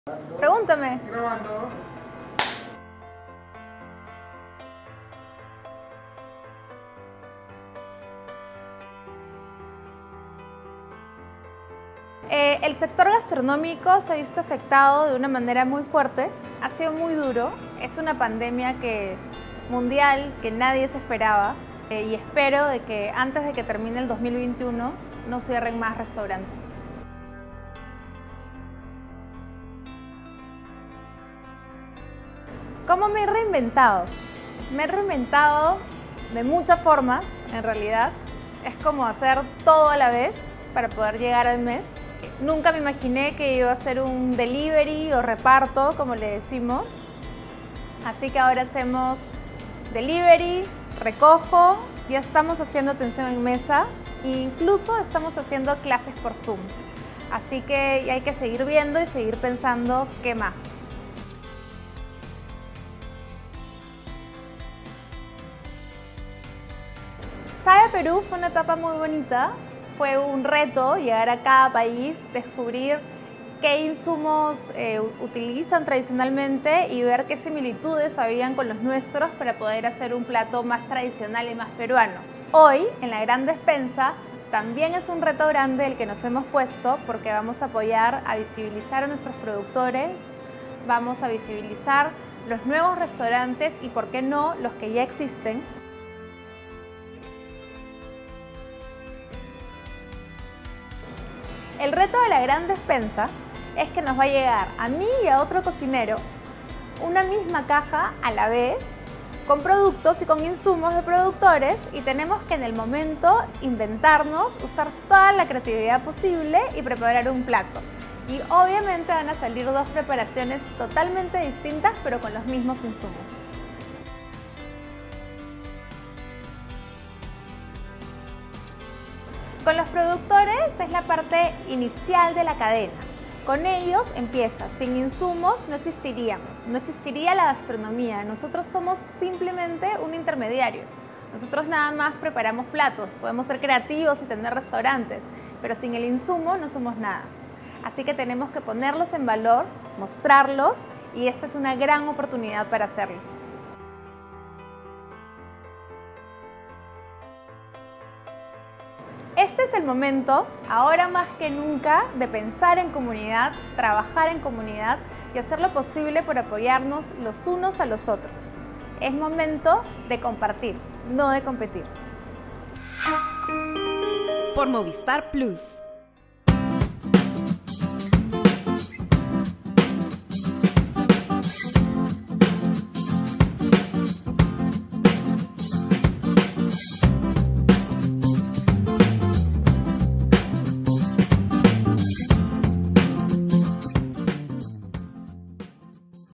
Entrevista a Arlette Eulert sobre su nuevo programa | La Gran Despensa
¡La gran despensa llega a Movistar Plus! 👏
Si quieres saber todo sobre esta nueva propuesta televisiva gastronómica, no lo pienses dos veces y dale play al vídeo, que Arlette Eulert nos cuenta todos los detalles. 😱